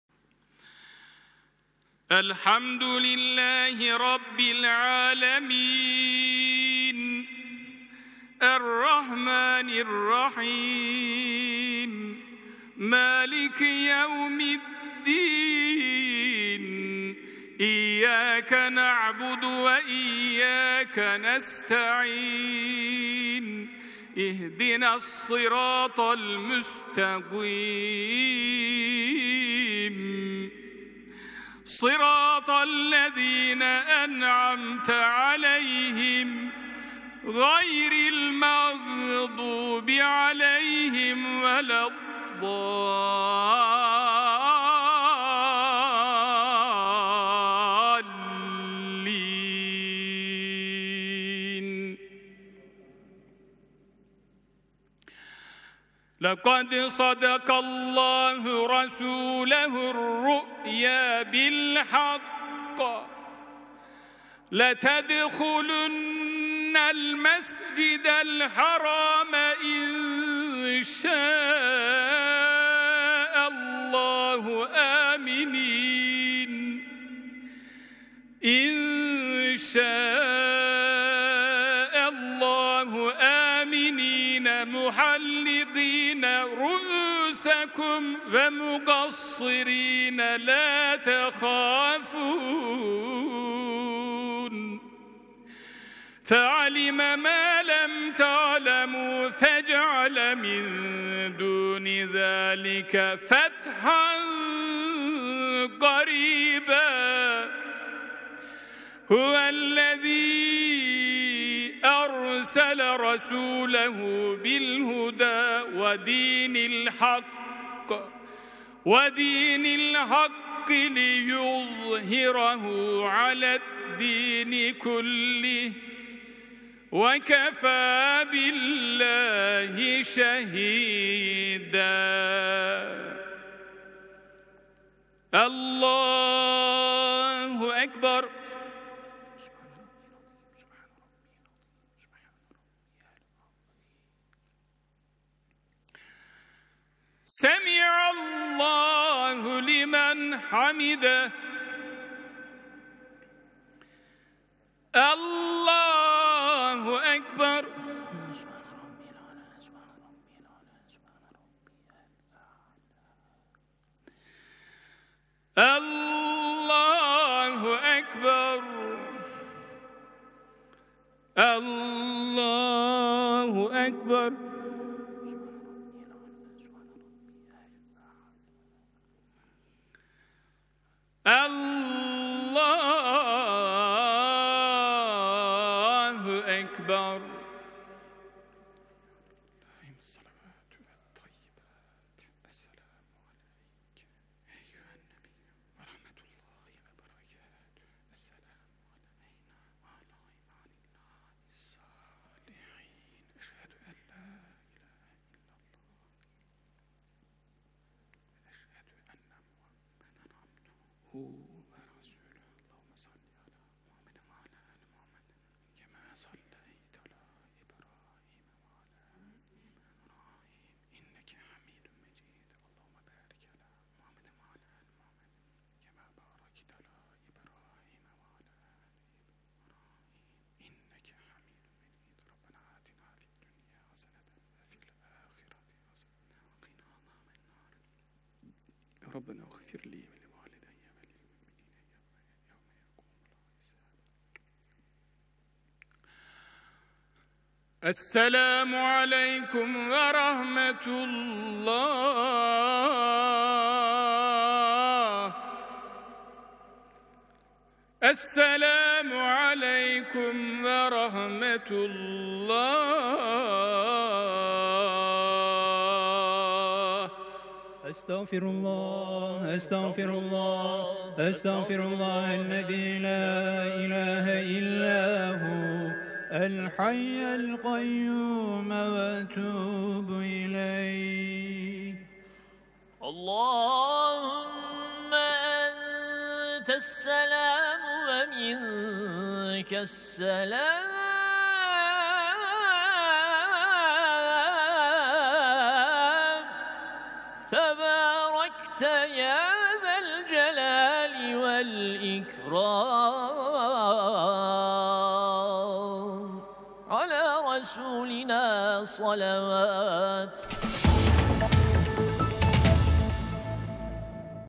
اقامه نخستین نماز جمعه در مسجد کبیر ایاصوفیه پس از 86 سال
نخستین نماز جمعه با جضور هزاران نفر پس از 86 سال در مسجد کبیر ایاصوفیه اقامه شد
# ایاصوفیه
# نماز جمعه